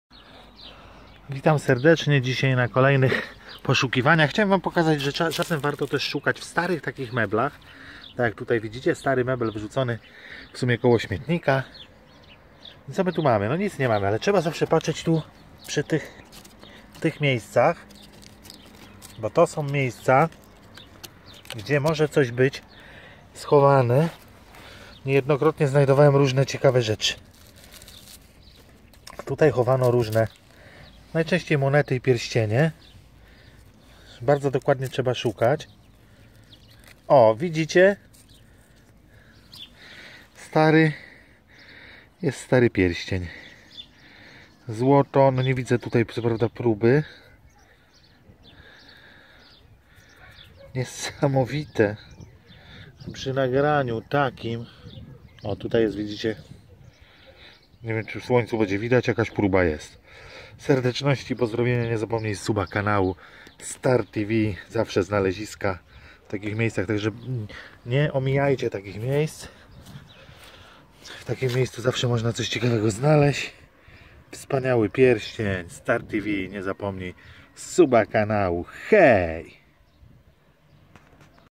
ZNALEZISKO ZŁOTY PIERŚCIEŃ W STARYM ŁÓŻKU NA ŚMIETNIKU
złoty pierścien znalezisko-wykopki skarby.Co można znaleźć w starym łóżku.Znalezisko wykopki na smietniku